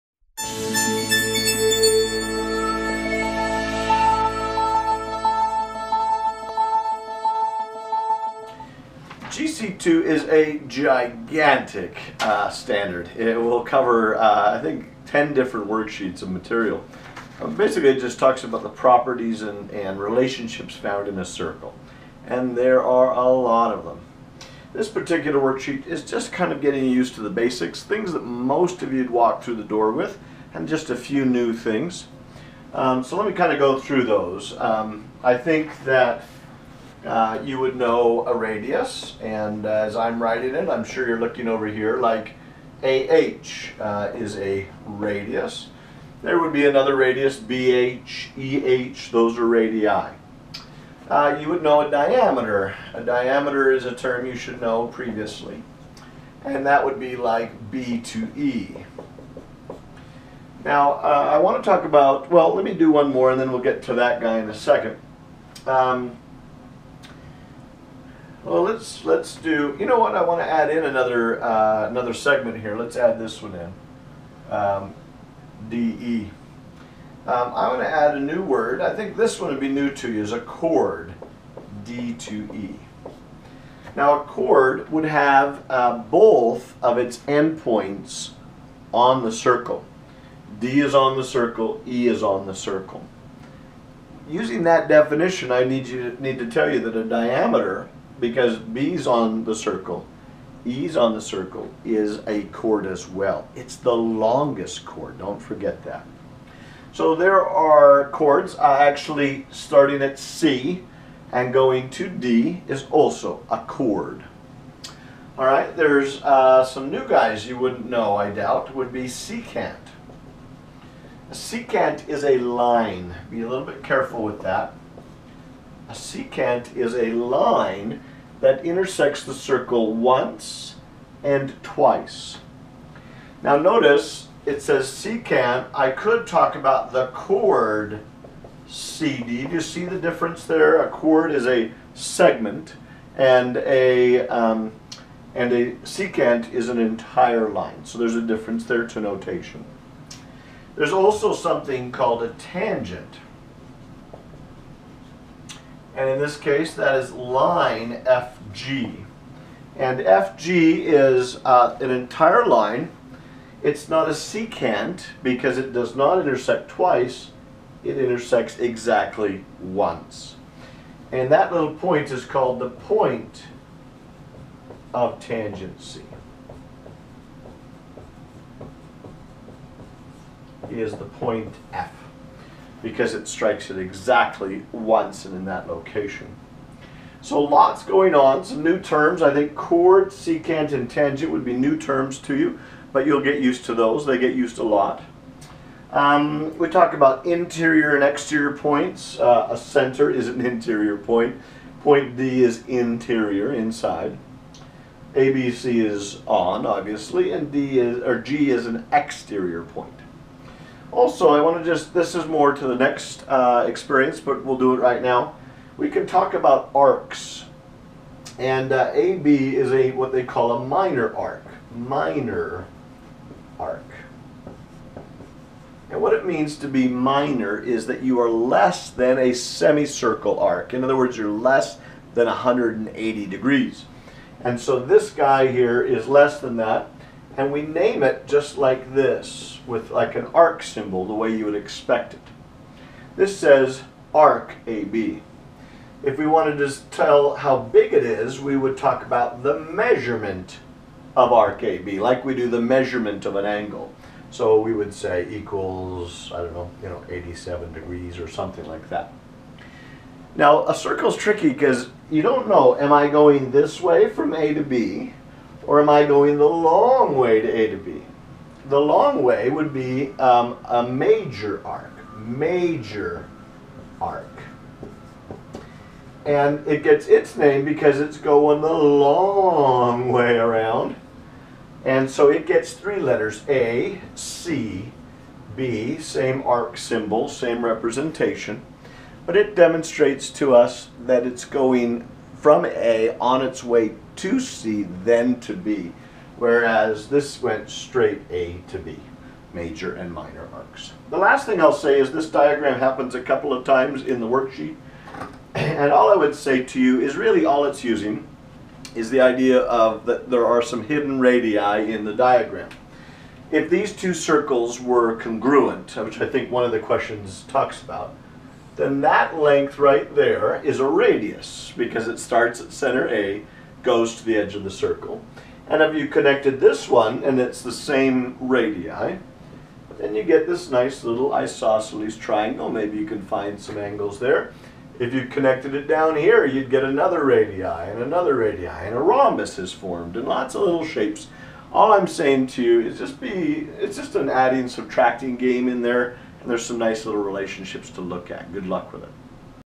G-C.A.2 Worksheet #1 - Circle Basics and Terminology
G-C.A.2 Worksheet #1 introduces the basics objects related to a circle.